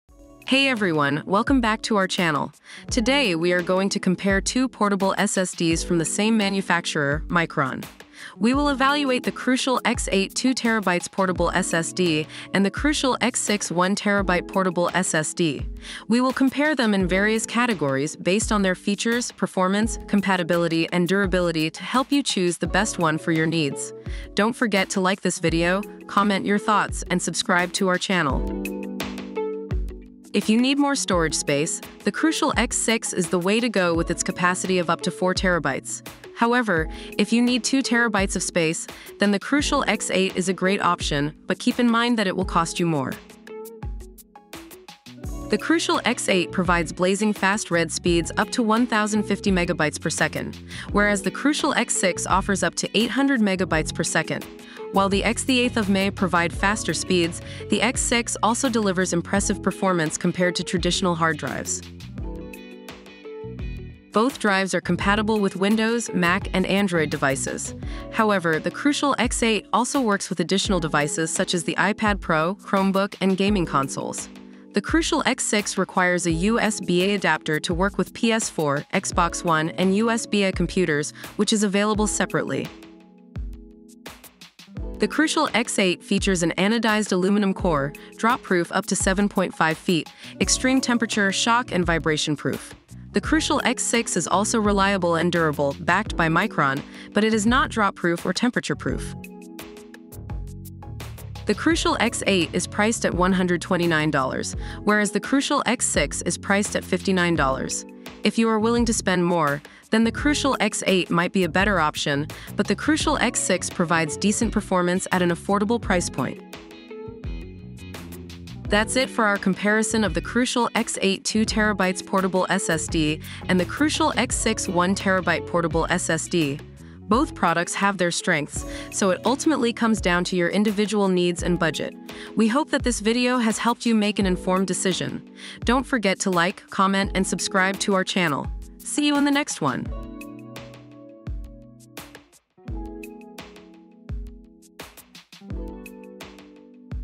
Crucial X8 vs. Crucial X6: Which Portable SSD is Right for You?
In this video, we compare the Crucial X8 and the Crucial X6 portable SSDs. Which one should you choose?